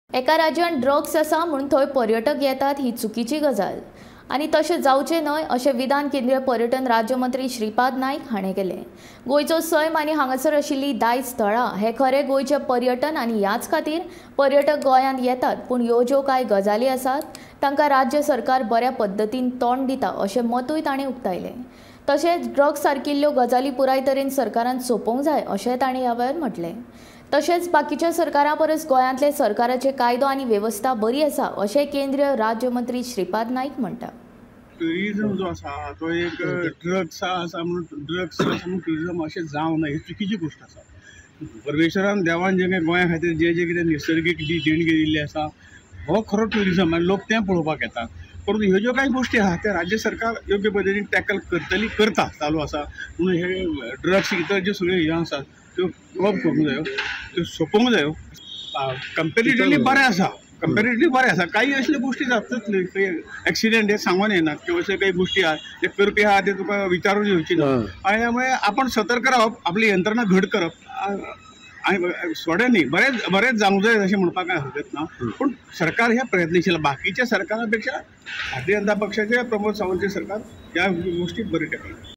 पर्यटकांनी गोव्यात निसर्गाचा आनंद घेण्यासाठी यावे - नाईक | Gomantak TV
Watch latest Goa News updates on Gomantak Tv here:
पर्यटकांनी गोव्यात निसर्गाचा आनंद घेण्यासाठी यावे - नाईक | Tourist should come in Goa to witness Nature - Naik | Gomantak TV